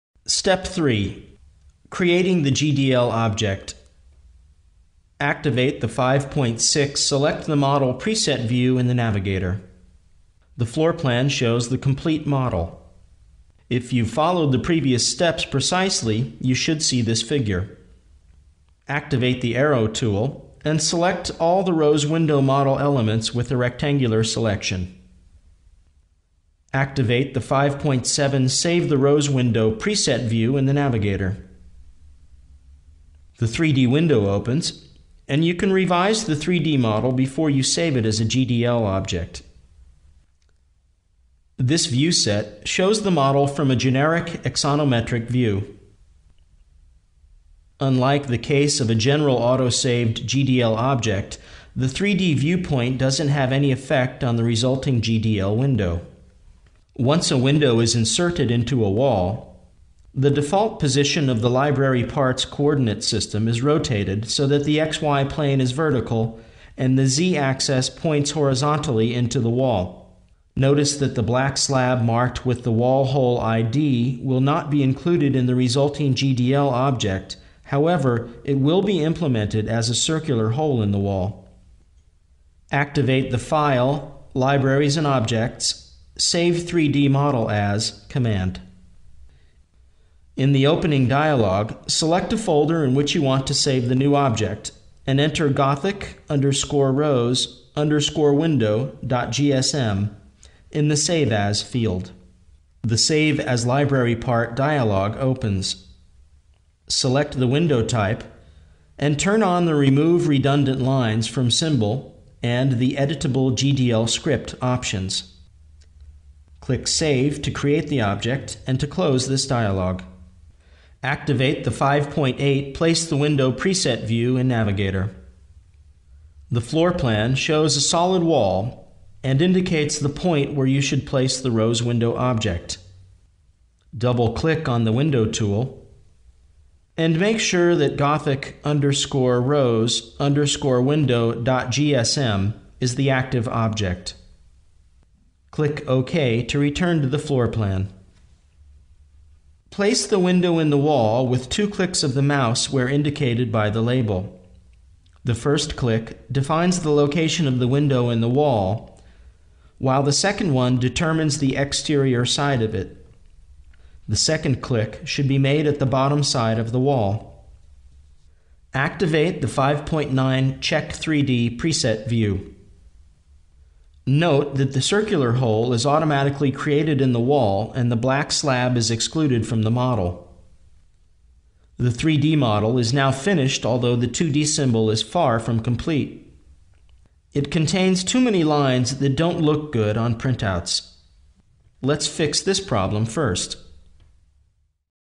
ArchiCAD Building Object Creation ITG 5-3
In this movie: Auto Saving Non-rectangular Windows - Creating the GDL Object